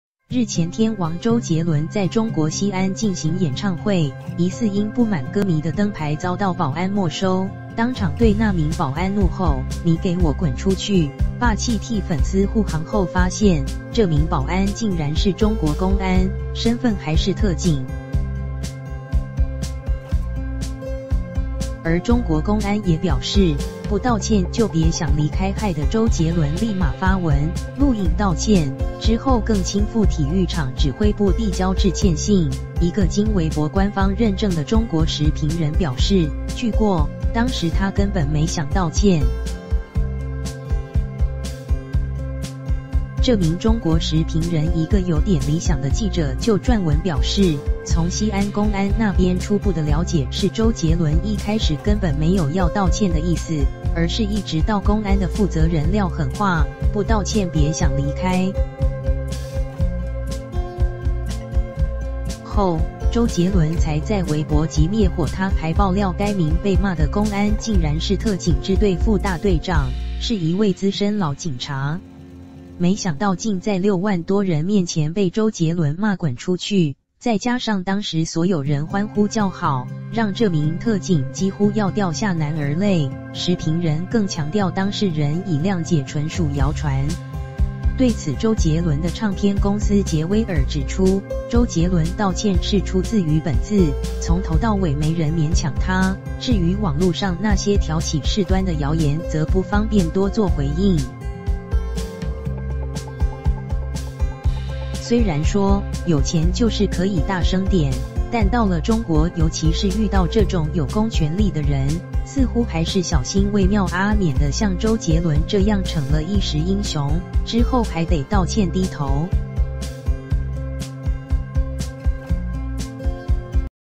周杰倫出事了！中國公安局負責人放重話：休想離開西安！！
日前天王周杰倫在中國西安進行演唱會，疑似因不滿歌迷的燈牌遭到「保安」沒收，當場對那名「保安」怒吼：「你給我滾出去！」霸氣替粉絲護航後發現，這名「保安」竟然是「中國公安」，身份還是特警。而中國公安也表示：「不道歉就別想離開。」害的周杰倫立馬發文、錄影道歉，之後更親赴體育場指揮部遞交致歉信，一個經微博官方認證的中國時評人表示：「據過，當時他根本沒想道歉。」

這名中國時評人「一個有點理想的記者」就撰文表示，從西安公安那邊初步的了解是周杰倫一開始根本沒有要道歉的意思，而是一直到公安的負責人撂狠話：「不道歉別想離開。」後，周杰倫才在微博急滅火...他還爆料該名被罵的公安竟然是特警支隊副大隊長，是一位「資深老警察」！沒想到竟在6萬多人面前被周杰倫罵「滾出去！」再加上當時所有人歡呼叫好，讓這名特警幾乎要掉下男兒淚，時評人更強調「當事人已諒解純屬謠傳」！對此周杰倫的唱片公司傑威爾指出：「周杰倫道歉是出自於本自，從頭到尾沒人勉強他，至於網路上那些挑起事端的謠言則不方便多做回應。」雖然說，有錢就是可以大聲點，但到了中國尤其是遇到這種有「公權力」的人，似乎還是小心為妙阿...免得像周杰倫這樣逞了一時英雄，之後還得道歉低頭。